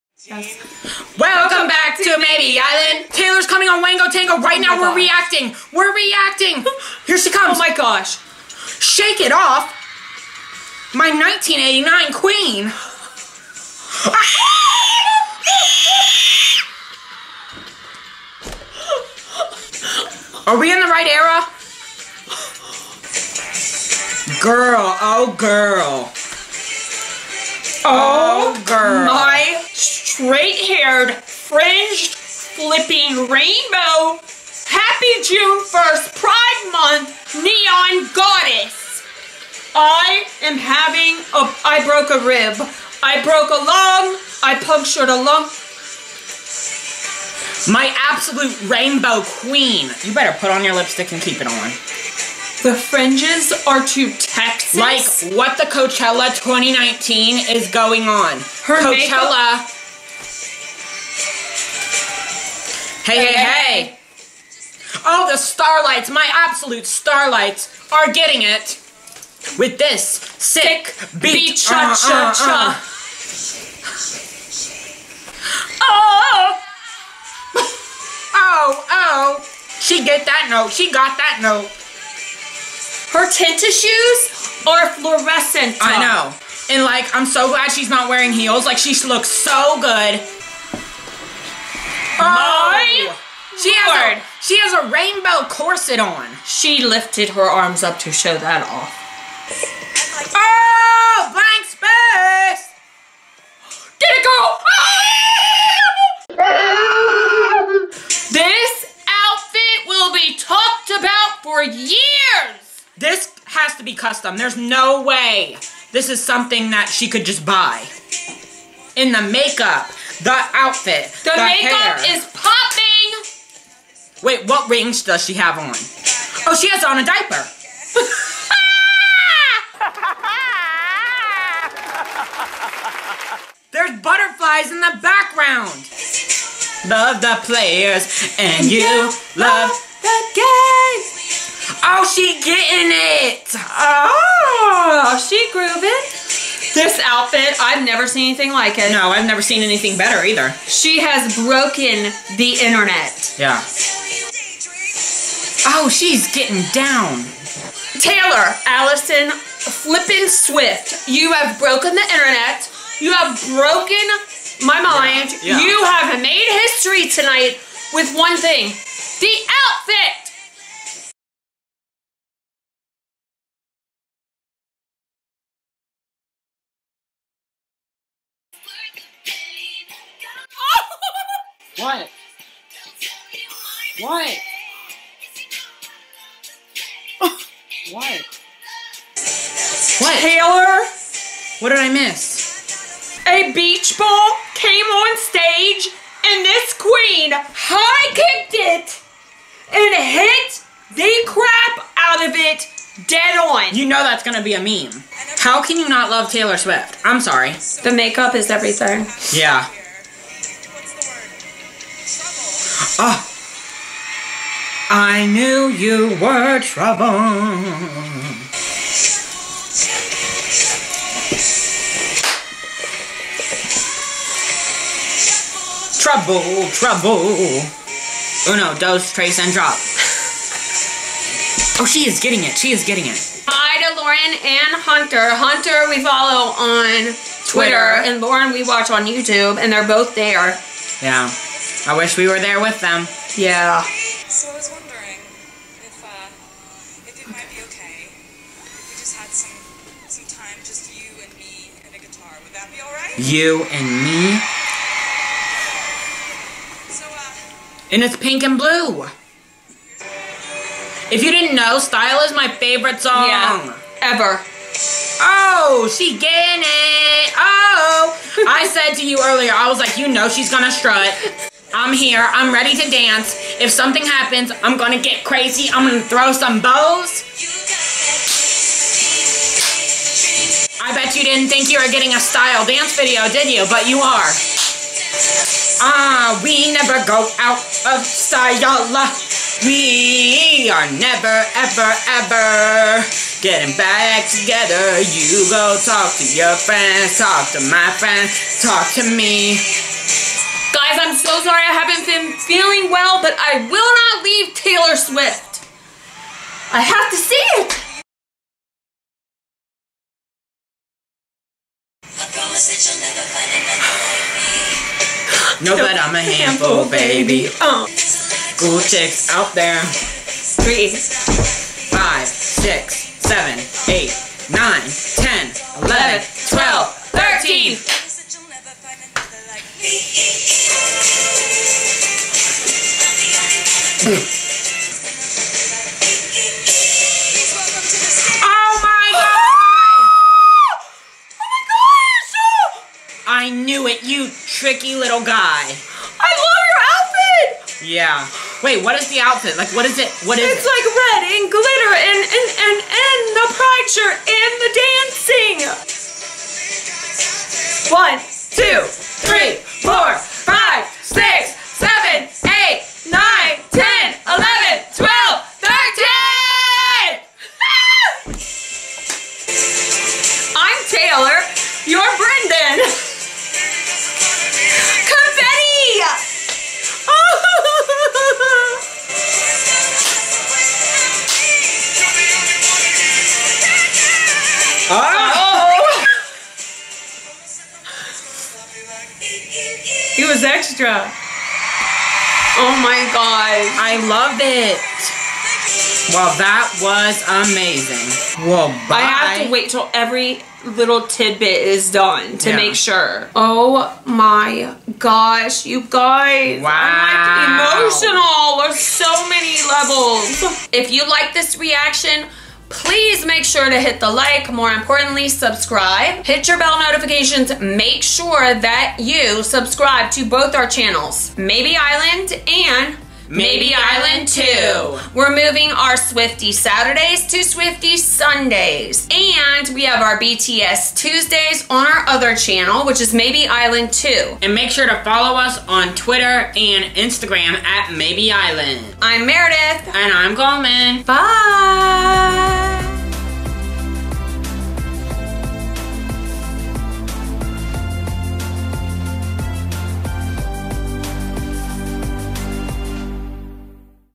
Taylor Swift Reaction | Wango Tango 2019 LIVE | AND I OOP-
We reacted to TaylorSwift at the 2019
AND I OOP- 🤯

All music and footage DOES NOT belong to us!

If you’re reading this comment “I’m an Islander now!"